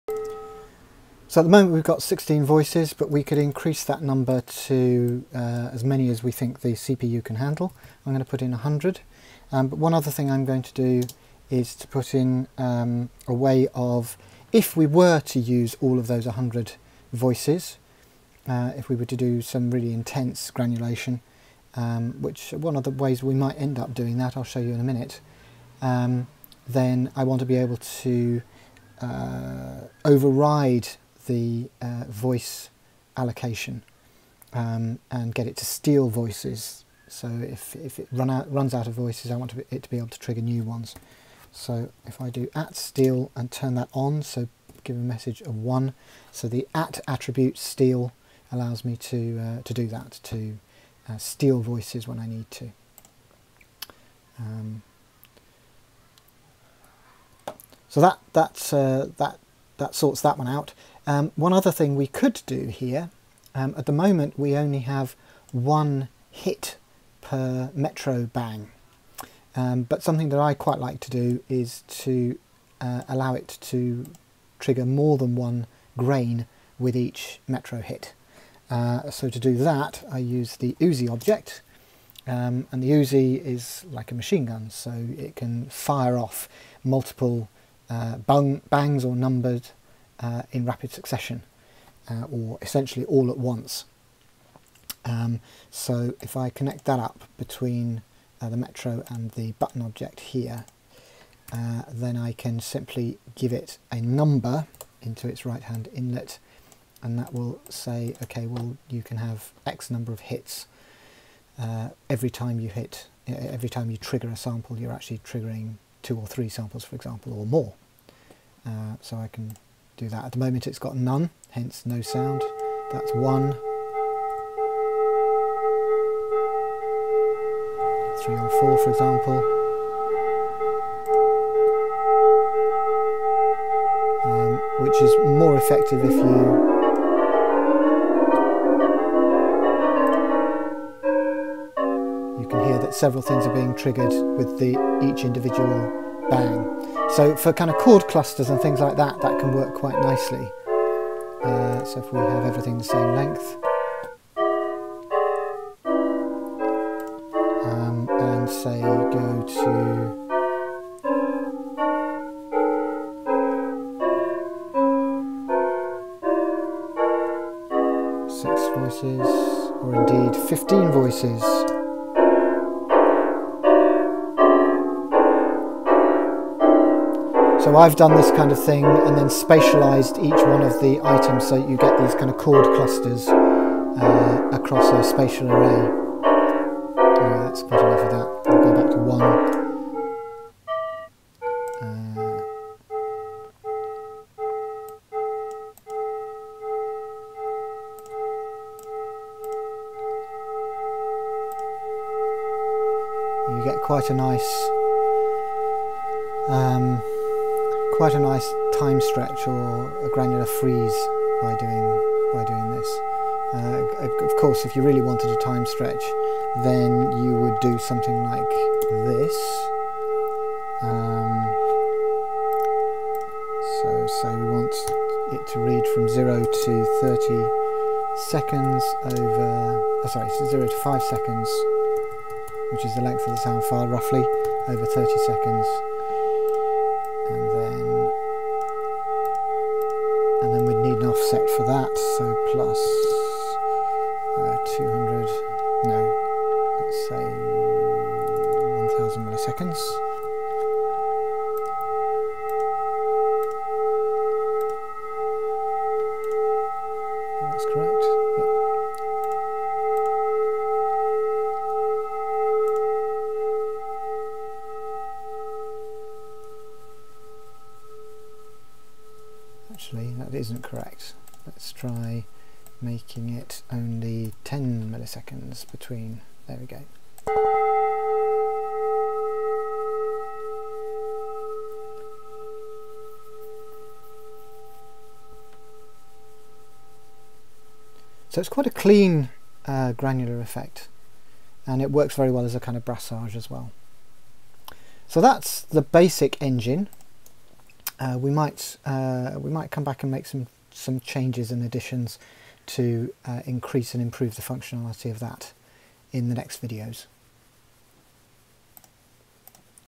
Plode -- Build a granulator in Max7: 08. Grain Clusters & Granular Time Stretch
Build a basic granulator in Max 7 in less than an hour. Quicker if you listen at 1.25 or 1.5 speed (greatly recommended: the ums/ers are less irritating). We'll then spend the next hour adding some bells and whistles.

We trigger multiple grains at once and explore time stretching.